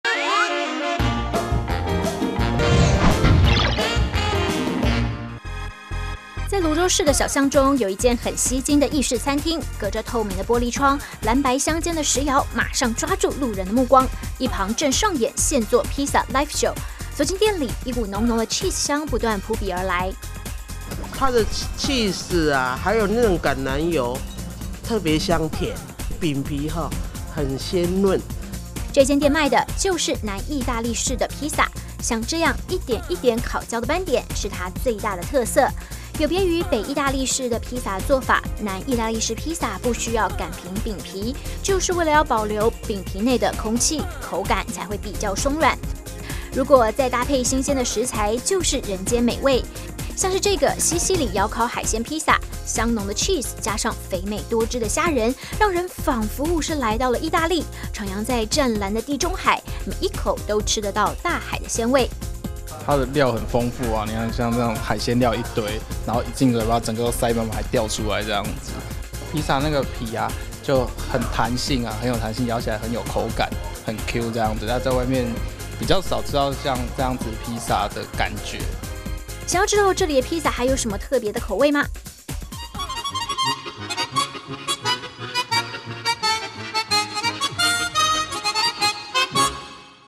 湯姆咖啡廚房
巧克力口味的比薩，竟然有蛋塔般綿密的口感！